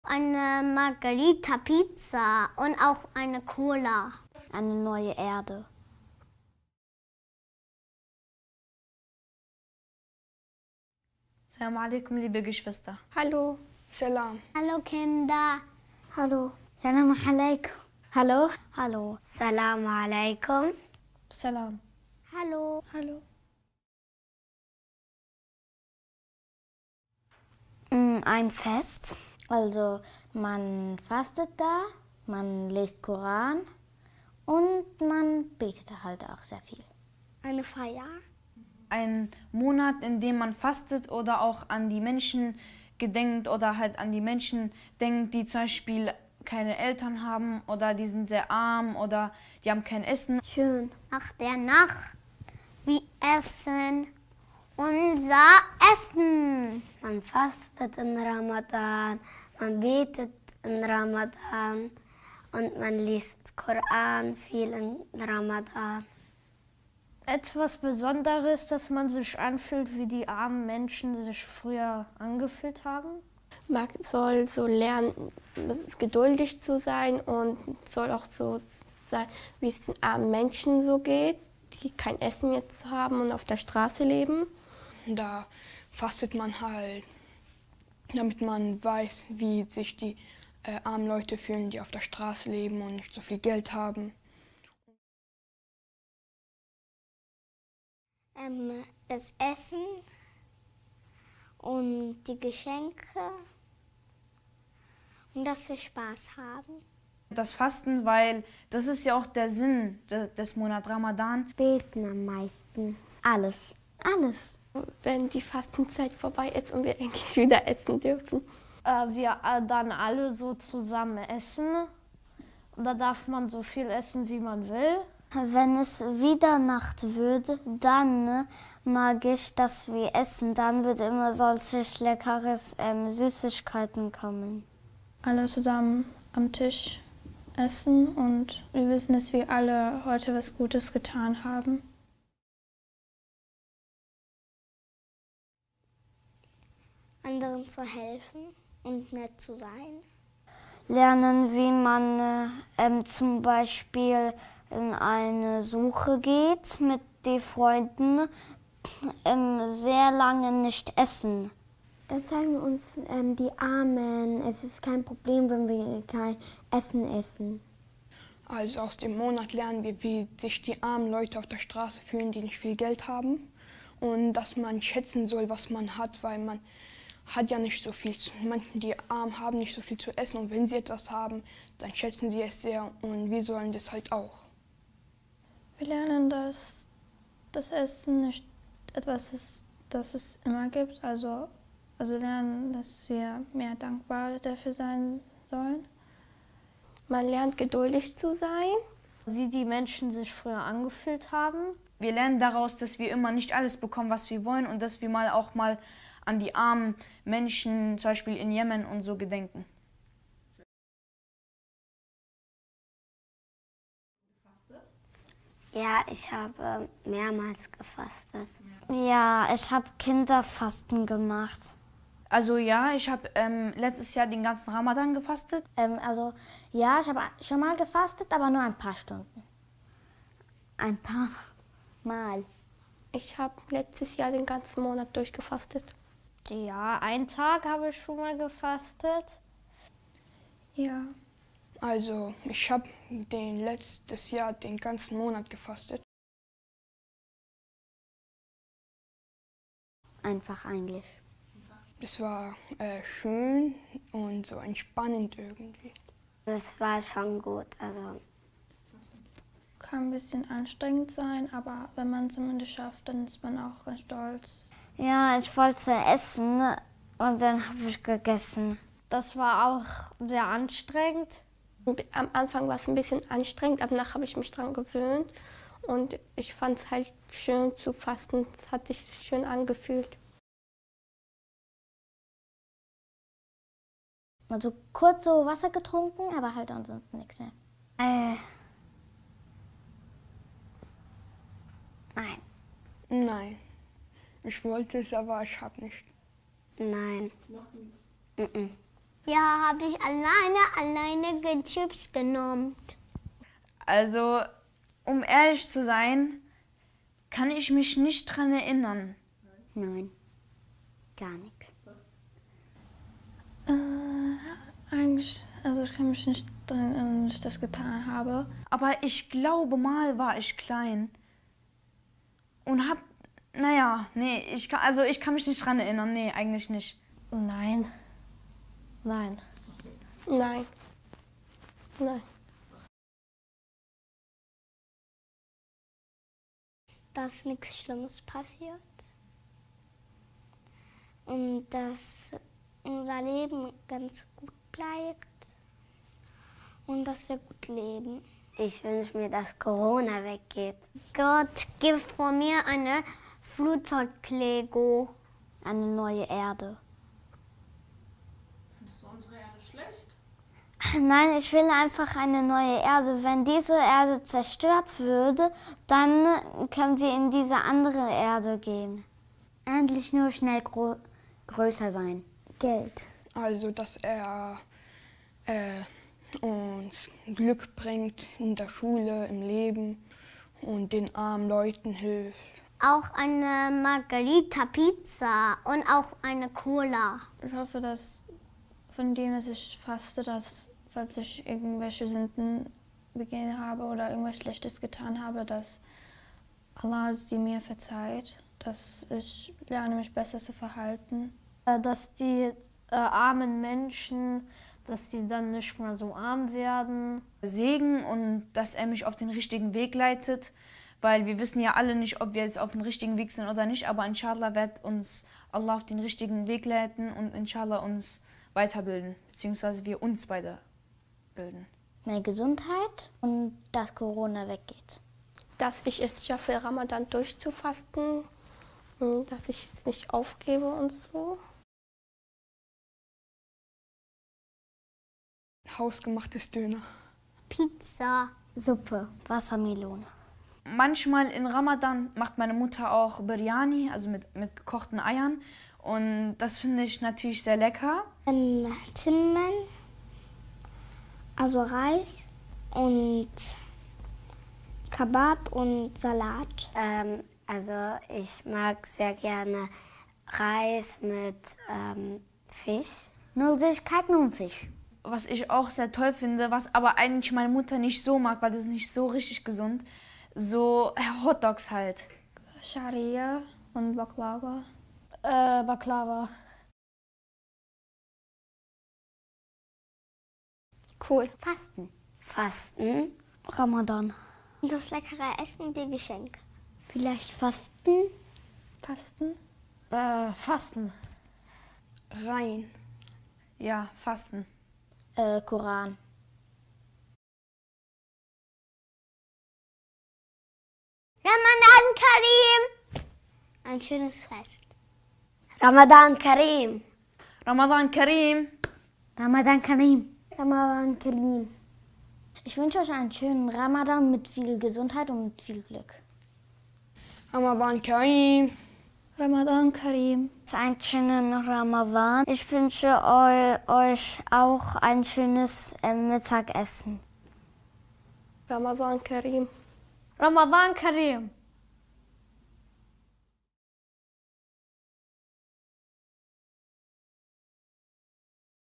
Kinder erklären den Monat Ramadan - Pizza Margherita& Cola
Ihr könnt uns gerne auf unseren sozialen Medien besuchen:
Wir wünschen euch einen gesegneten Monat Ramadan!

00:00- 00:23 Was ist der Monat Ramadan?
00:23- 01:35 Was gefällt dir am Monat Ramadan am meisten?
01:35- 02:23 Was lernen wir von dem Monat Ramadan?
02:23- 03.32 Hast schonmal ein Paar Stunden gefastet?
03:32- 04:06 Wie war´s?
04:06- 04:41 Hast du schonmal heimlich gegessen?
04:41- 05:32 Was wünschst du dir von Gott in dem Monat?
05:32- 07:12 Was ist dein Lieblings Iftar/Essen?
07:12- 07:57 Beschreibe den Monat Ramadan in einem Wort:
07:57- 08:17 Wir wünschen euch allen: